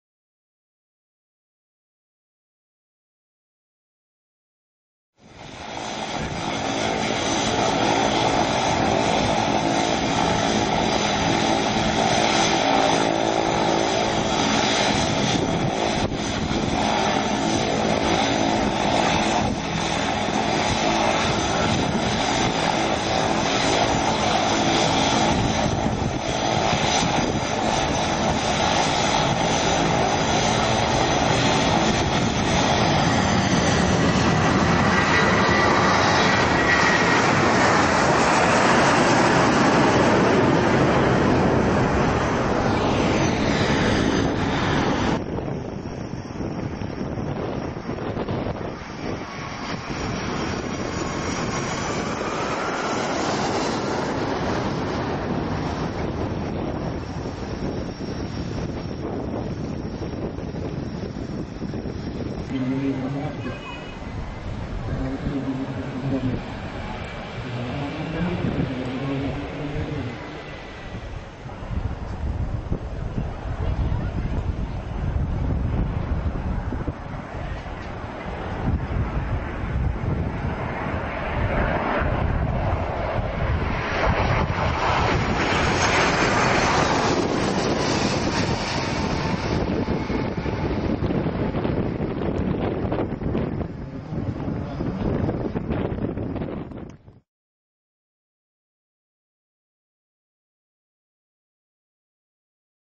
Boeing 757 Short Field Takeoff
A Boeing 757 short field takeoff at an airshow.  Pretty impressive!